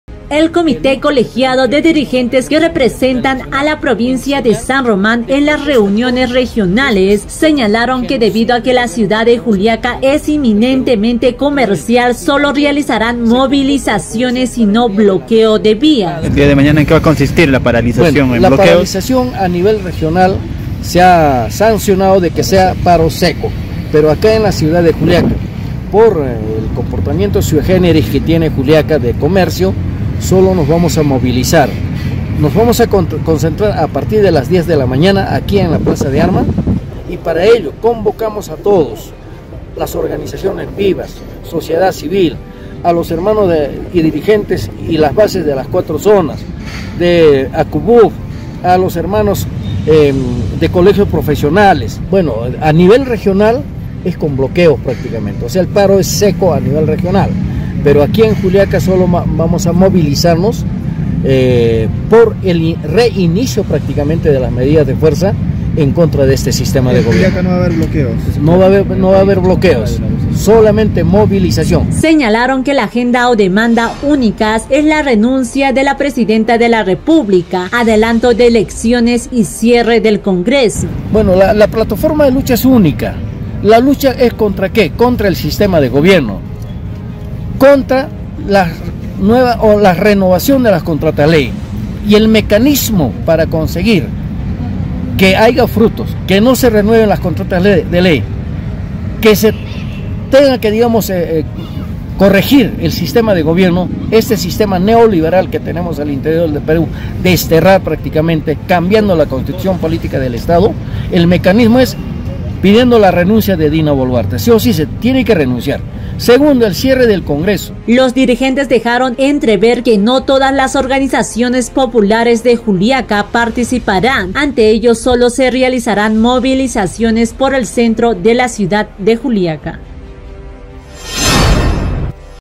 PARO: DIRIGENTES DESCATARAN PARTICIPACIÓN EN PARO SECO
DIRIGENTES DE LA CIUDAD DE JULIACA DESCARTARON QUE ESTE MARTES DE 30 DE MAYO SE BLOQUEEN VÍAS DE COMUNICACIÓN, SIN EMBARGO, ANUNCIARON MASIVAS MOVILIZACIONES EN JULAICA